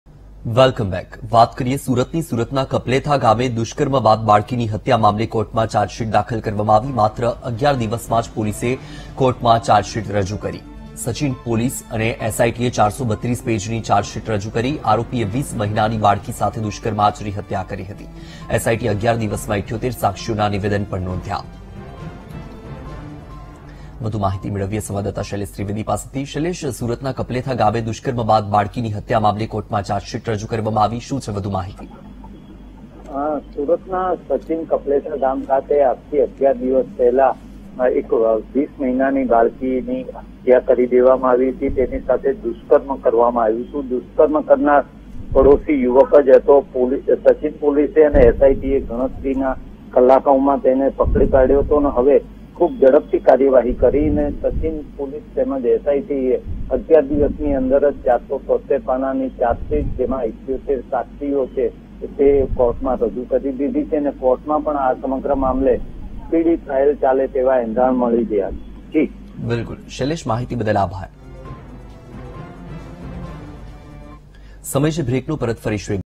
Surat News: કપલેથા ગામમાં દુષ્કર્મ બાદ 20 મહિના ની નાનકડી છોકરીની હત્યા
Surat News: પોલિસએ માત્ર 11 દિવસમાં કોર્ટમાં ચાર્જશીટ રજૂ કરી | VTV Gujarati